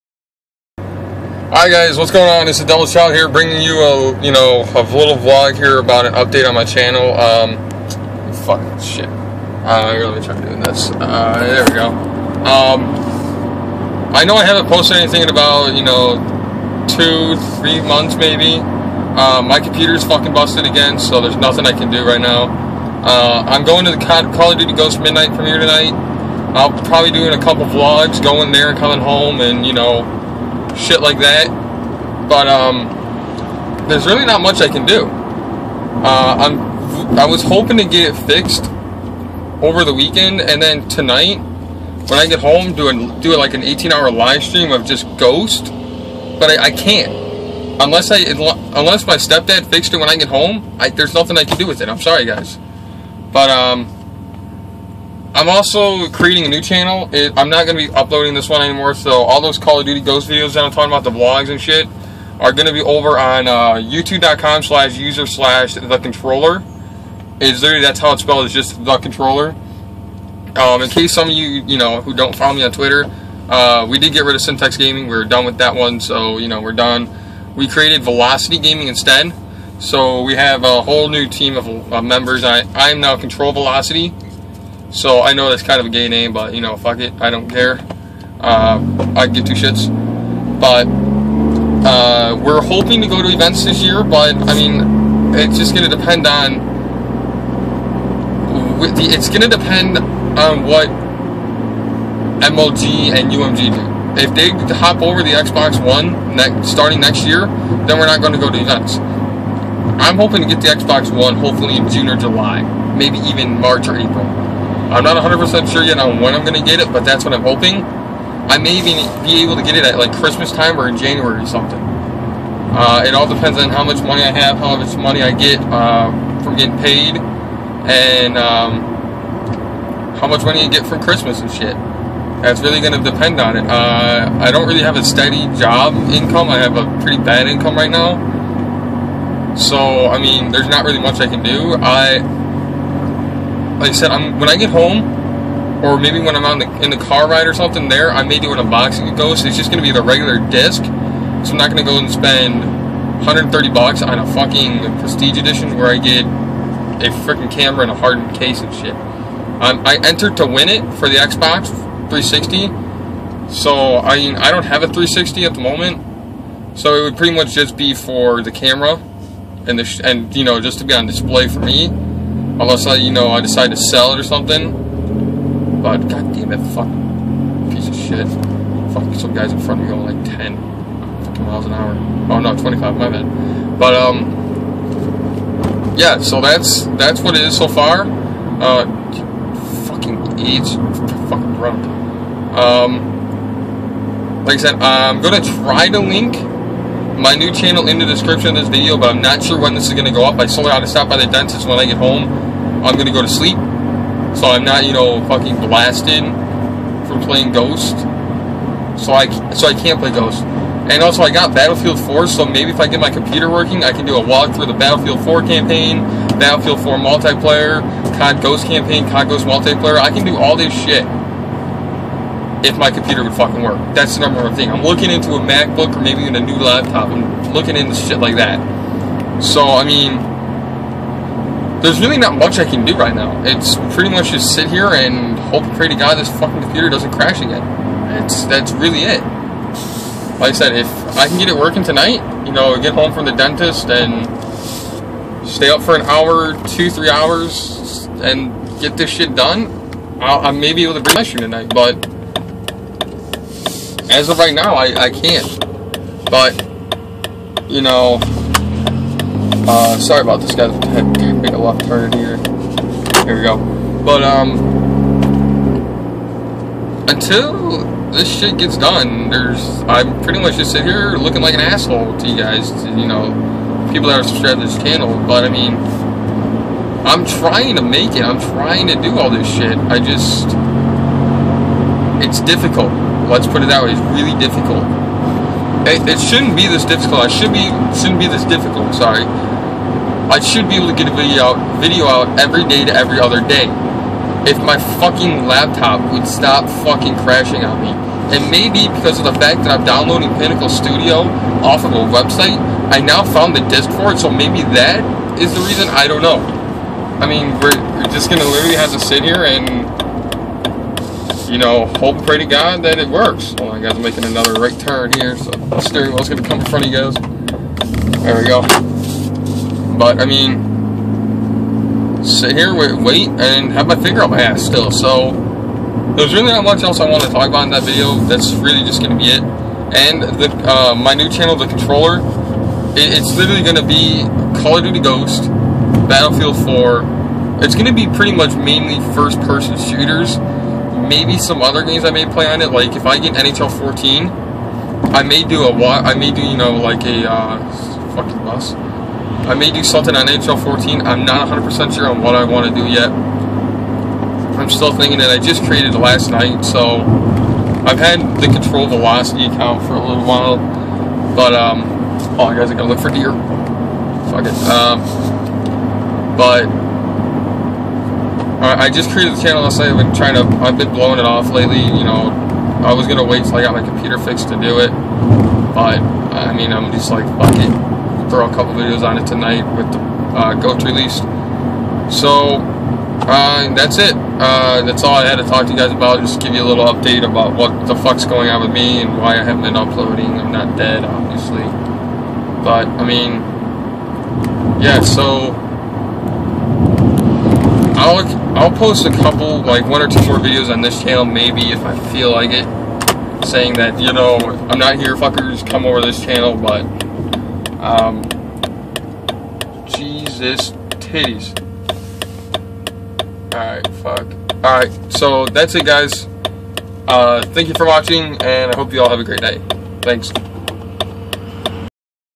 Update vlog new channel!!
Hey everyone its devil here bringing you a vlog just letting you know my new channel and everything thanks